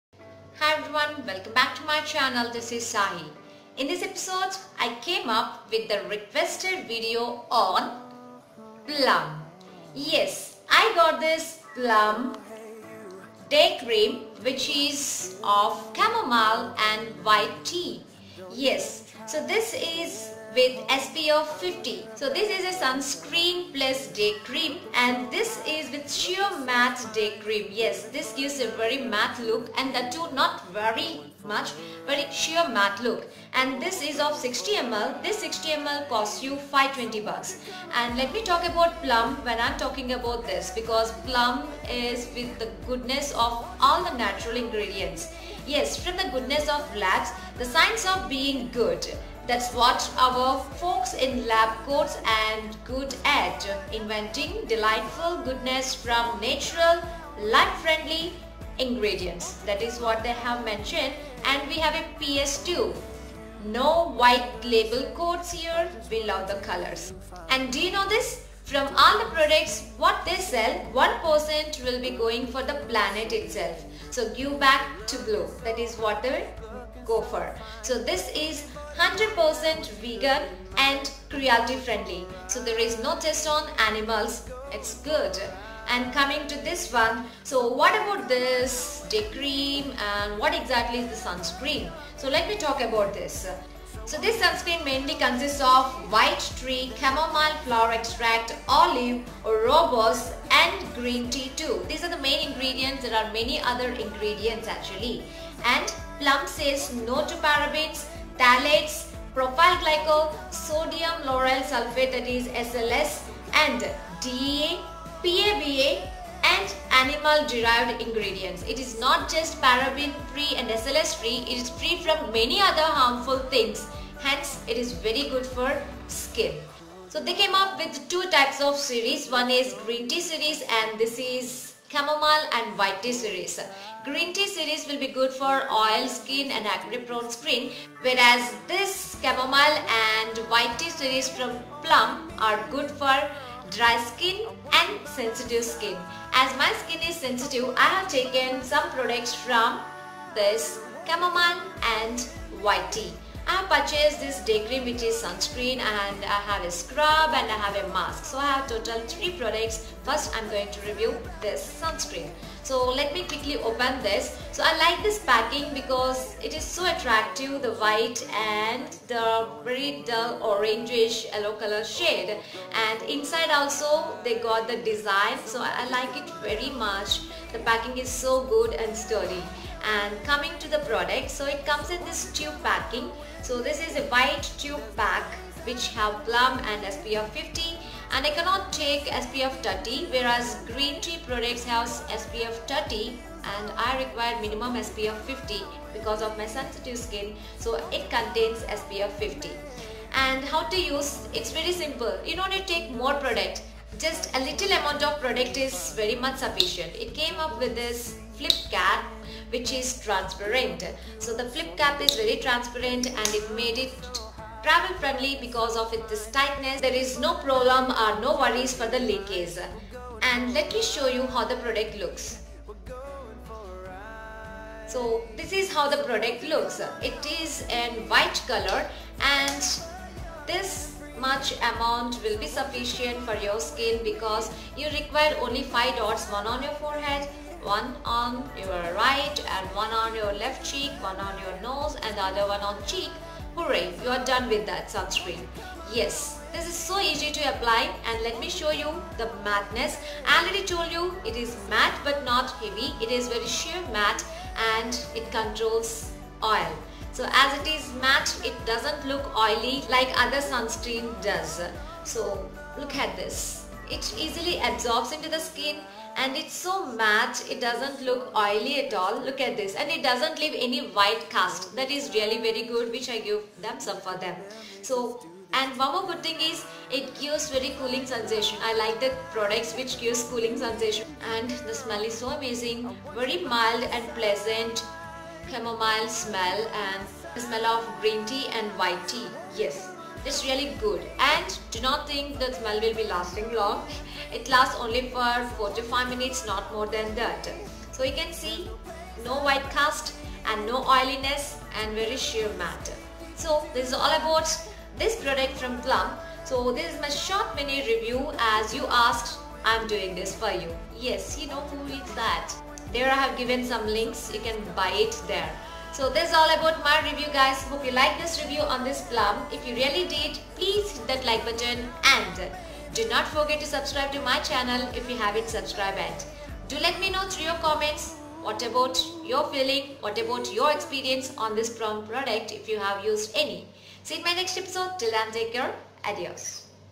Plum Goodness SPF 50 Chamomile & White Tea Day Cream Review | Skin Care Tips | SahiJeeth
Hi guys...
In this episode, I came up with one of the requested reviews on Plum Goodness SPF 50 Chamomile & White Tea Day Cream.
Am I using the right sunscreen for my skin type? Is it offering full range protection? Should I change to one that is less patchy? The process of choosing a sunscreen (or changing your current one) may be a long and confusing one. But we boil it down to 5 solid tips that will ensure you pick your sunscreen like a pro-every time!

Plum is noted for : Delightful, safe, paraben-free, phthalate-free, SLS-free skincare with rich natural ingredients like sea buckthorn, aloe, green tea, macadamia, chamomile.

Watch my video for complete information.
Like the video if you really did.